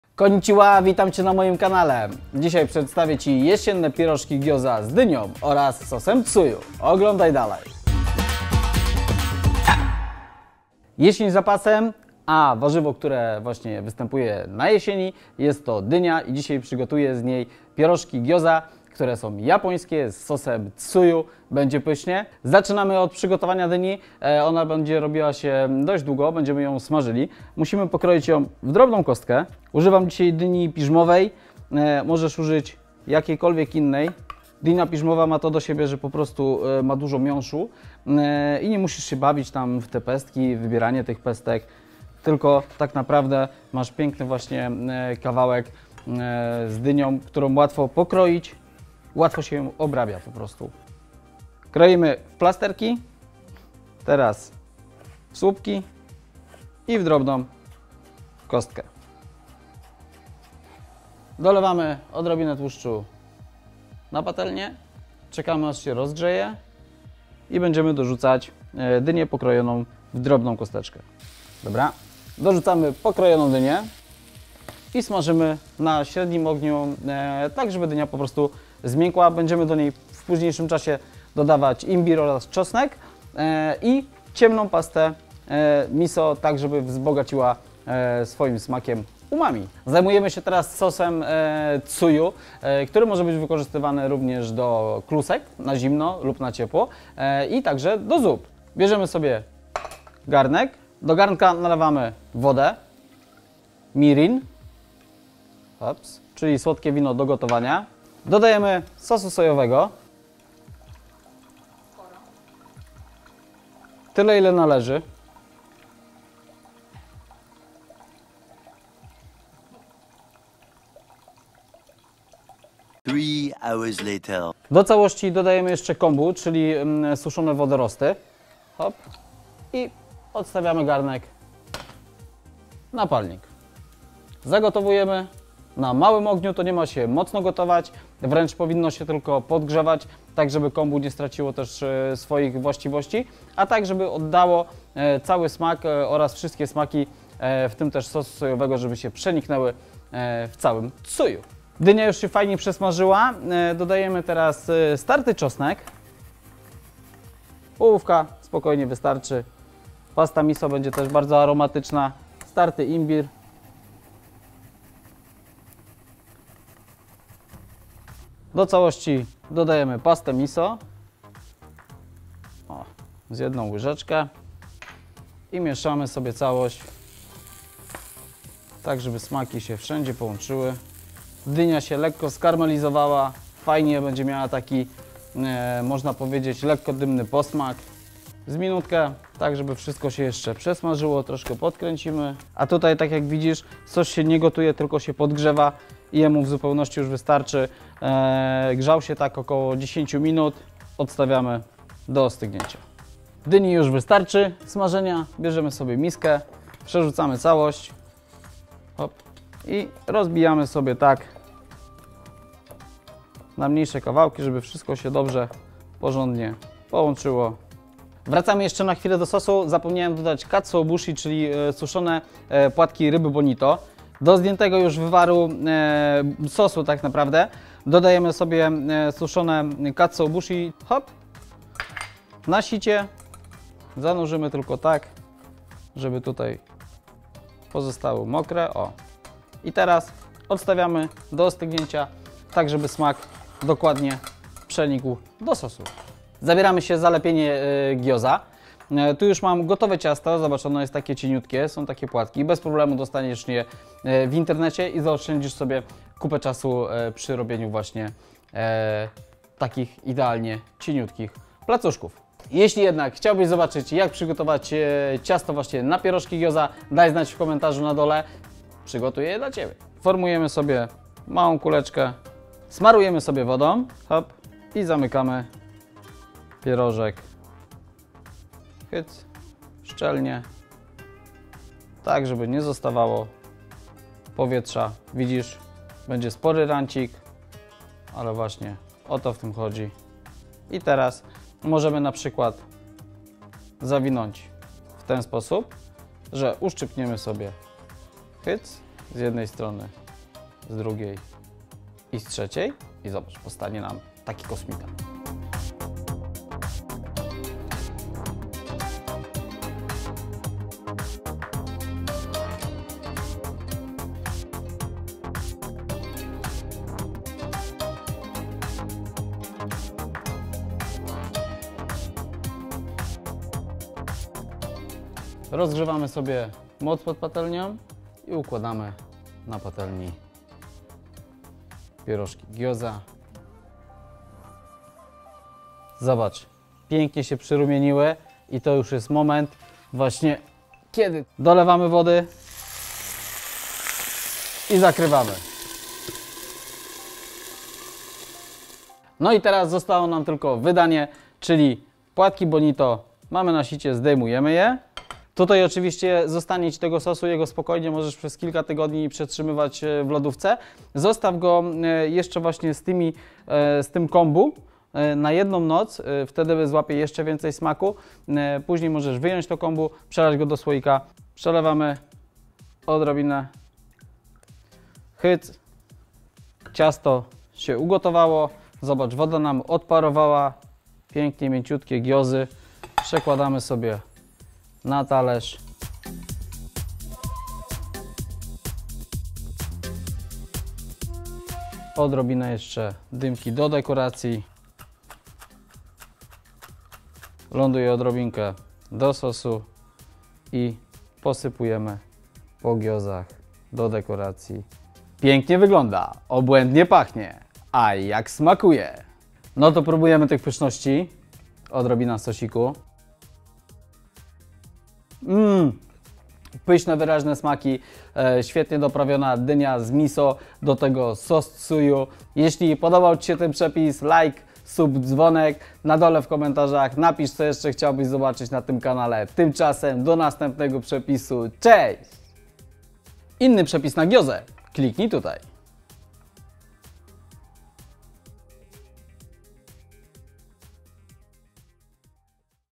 Japońskie PIEROŻKI GYOZA! Przepis z dyniowym farszem! | UMAMI
Dzisiaj zapraszam do Japonii i po kolejny przepis na pierożki, w końcu pierożków nigdy za wiele :)
Tym razem to przepis na japońskie pierożki gyoza z dyniowym farszem, a do tego sos tsuyu na bazie sosou sojowego. Tym razem użyłem gotowego ciasta, które możecie kupić online. Jeśli chcielibyście, żebym pokazał w osobnym odcinku jak zrobić takie ciasto, dajcie znać w komentarzach!

Daniel Brzeziński || UMAMI

Składniki:
12 płatków ciasta gyoza
200 g dynia piżmowa
Kolendra
Dymka
1 łyżka ciemnego miso

Sos tsuyu

50 ml Sake
100 ml mirin

120ml woda
10 g katsuobushi
10 g kombu